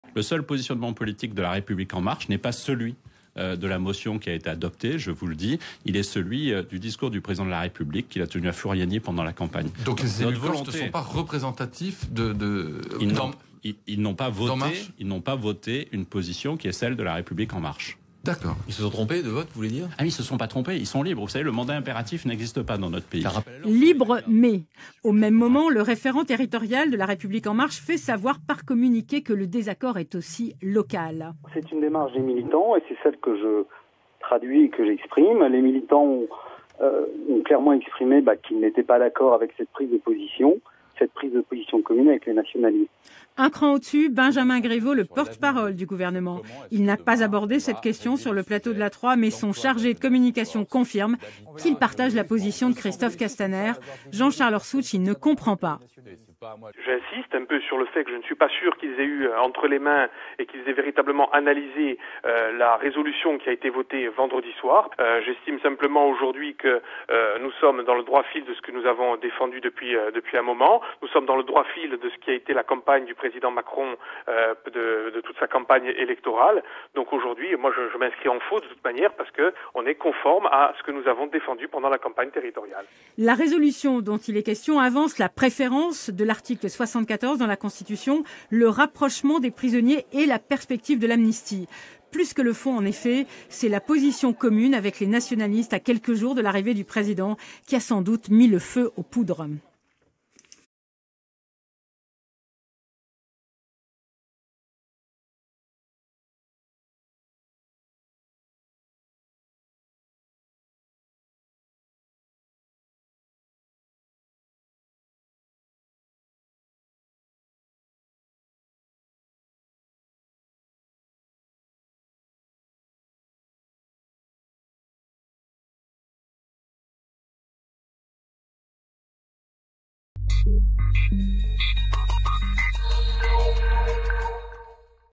Assemblée de Corse : Christophe Castaner désavoue les élus La République en marche
Reportage : ANTOMARCHI Florence ; BARBOLOSI Frederique ; BURESI Véronique.